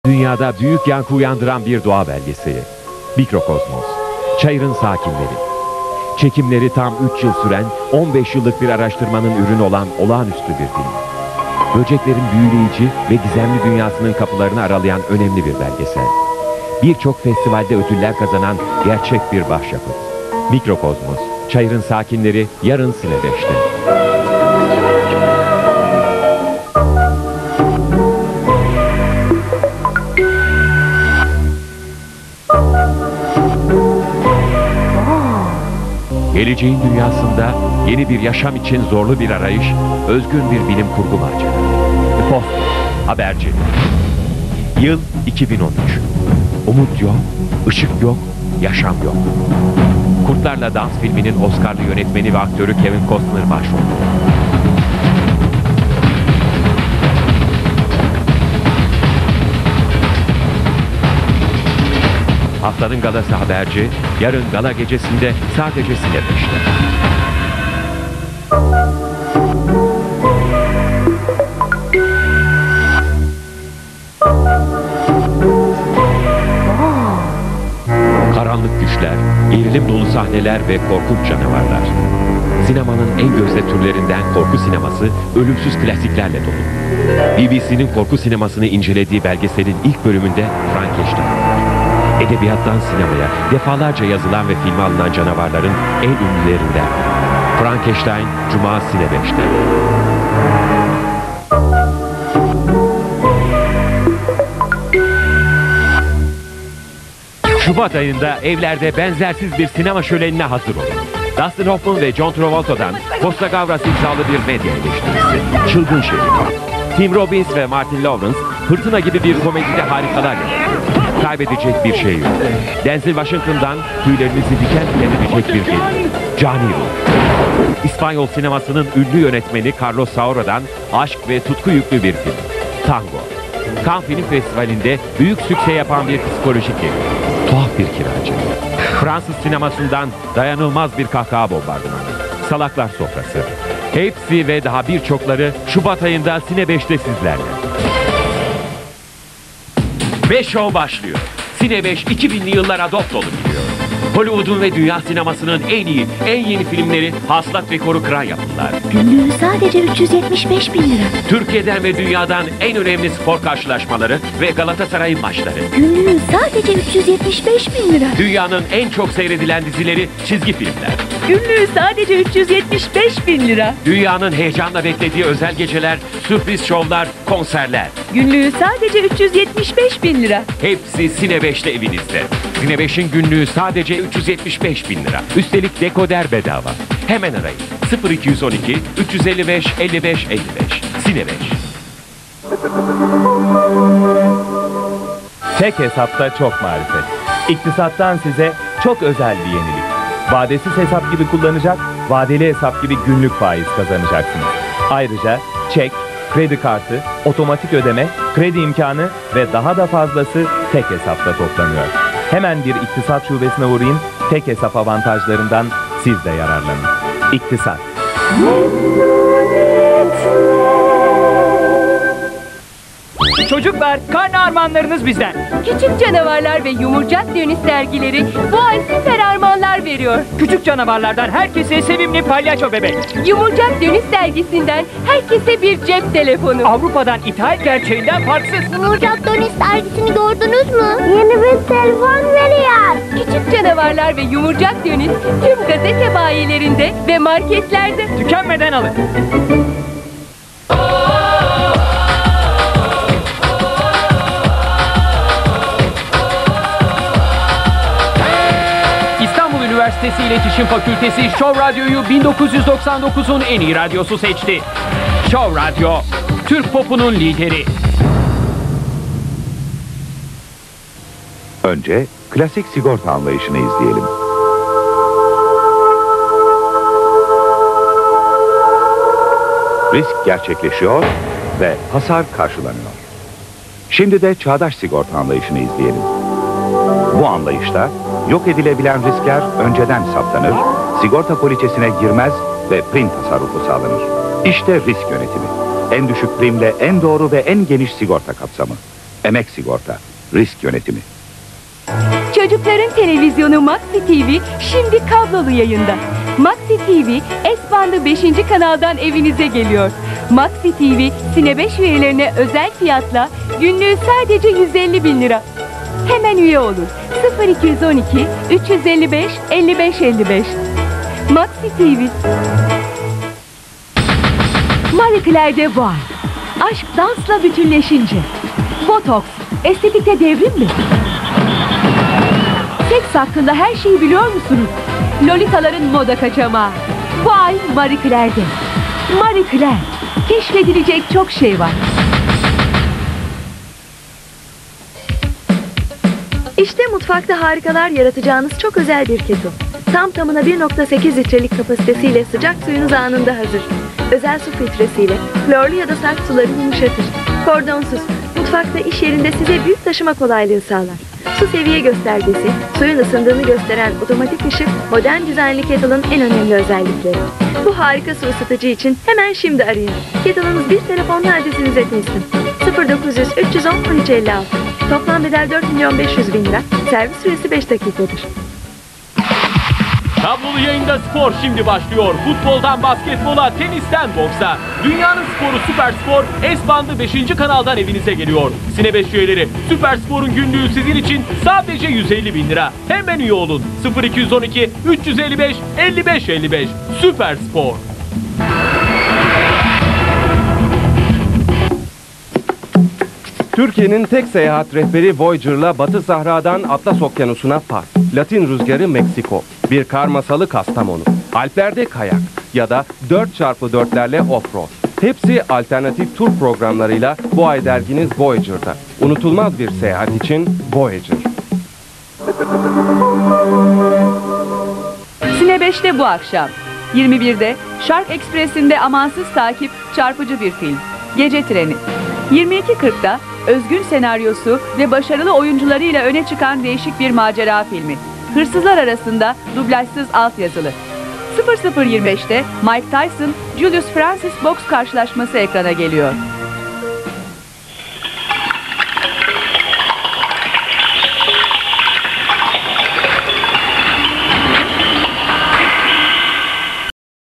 CINE5 Tanıtım-Reklam Kuşağı
Cine5 ekranlarında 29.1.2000 yılında ekrana gelen tanıtım ve reklam kuşağı. TV arşivcileri için...